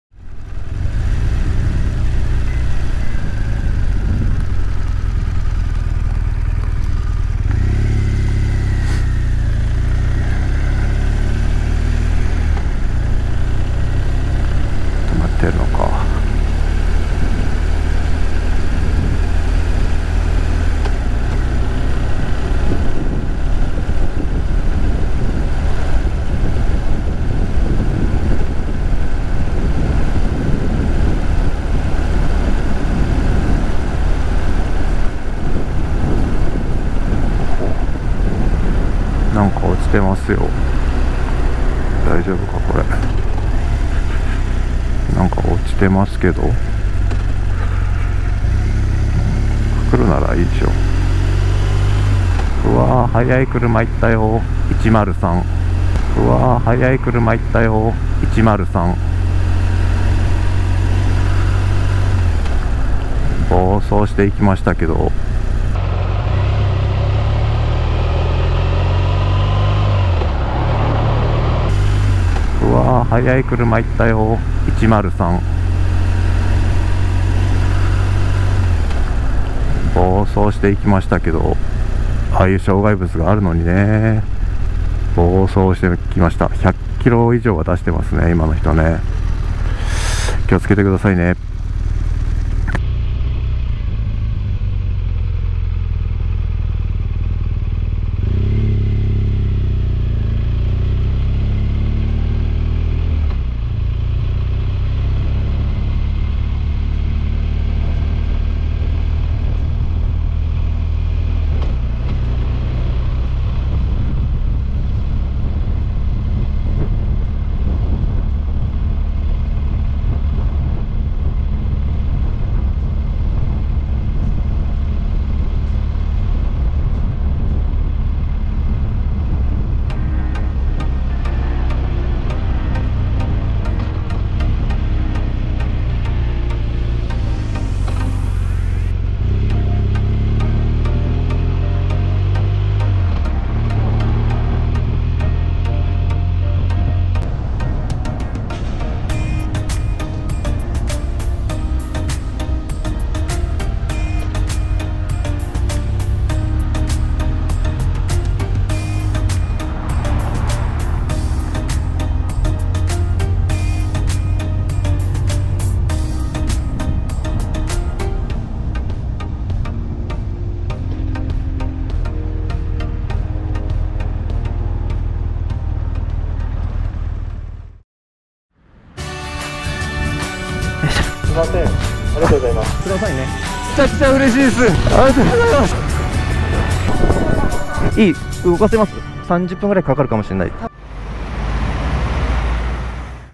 🟥赤キップだろ、コレ😰 💥暴走アウディには届かない警告 🫥MG I think it's a red kip.
💥『赤キップ』になると裁判が行われて、前科が付きます。前科が付いて社会生活を失うこともある非常に重い交通違反です🫡
⚠️後ろから猛スピードで来るアウディに、障害物の存在を知らせても、徒労に終わるだけでした😥
⚠️Informing him of the existence of the obstacle was just a waste of time.

About English translation
I'm not good at translating, but I hope you can understand the meaning of this video.